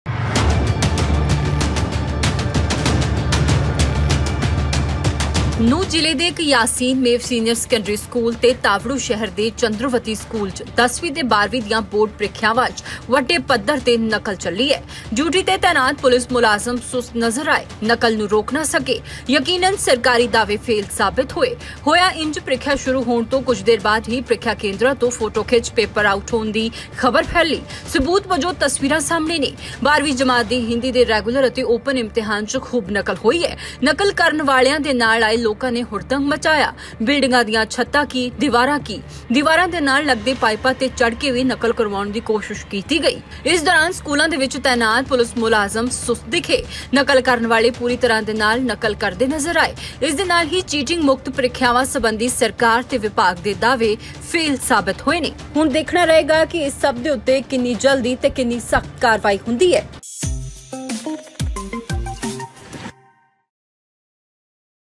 Nuh Exam Cheating|10ਵੀਂ ਅਤੇ 12ਵੀਂ ਦੀਆਂ ਬੋਰਡ ਪ੍ਰੀਖਿਆਵਾਂ 'ਚ ਵੱਡੇ ਪੱਧਰ 'ਤੇ ਚੱਲੀ ਨਕਲ
Large scale cheating in 10th and 12th board exams